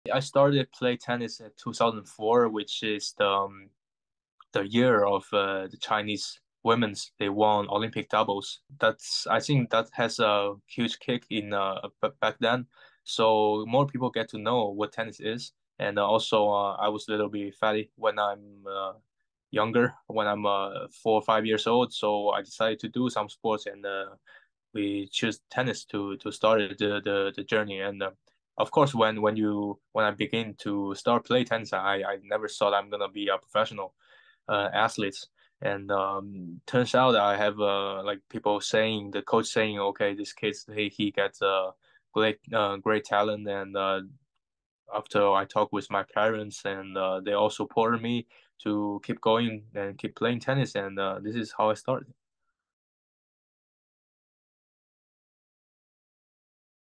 Wu Yibing on how he got into tennis
Wu Yibing started playing tennis to get in shape.

Now he does it to win medals.

In an exclusive interview with CGTN Europe, China's men's number one explains how it all started as a small boy in Hangzhou.